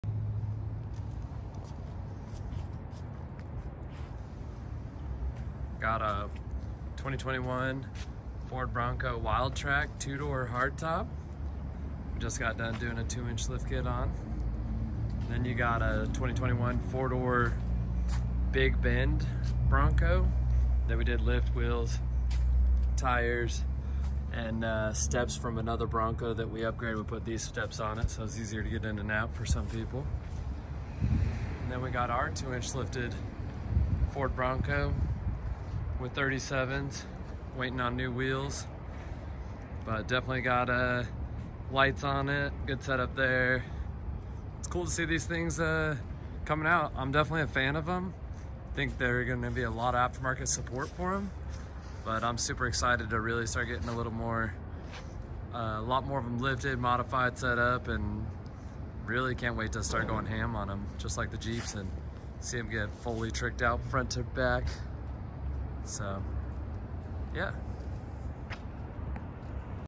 New Ford Bronco’s stacking up at the shop!
We got 3 New Ford Bronco’s at the shop all with a little different taste. The red one has plans to grow up more over the next few weeks. A quick look at 3 bronco in a parking lot that’s usually filled with Jeeps. We see these new Bronco’s being a big hit in the off road community. So Dixie has jumped in at full speed to keep current on these bad boys! Our St George store always has cool projects going on.